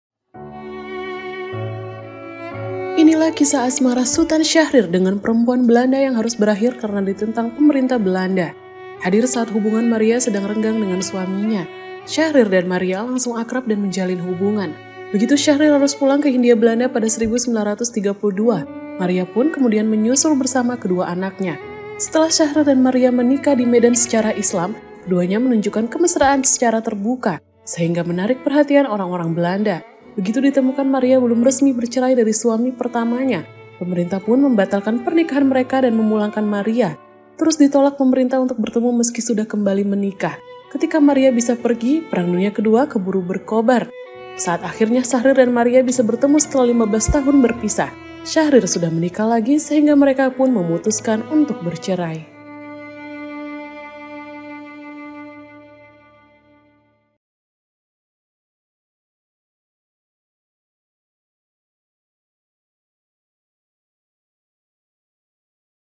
Akhir Pernikahan Sjahrir | HISTORIA.ID
Meski telah pindah agama, Maria tetap tidak diterima. Pernikahannya dengan Sutan Sjahrir dibatalkan pemuka agama.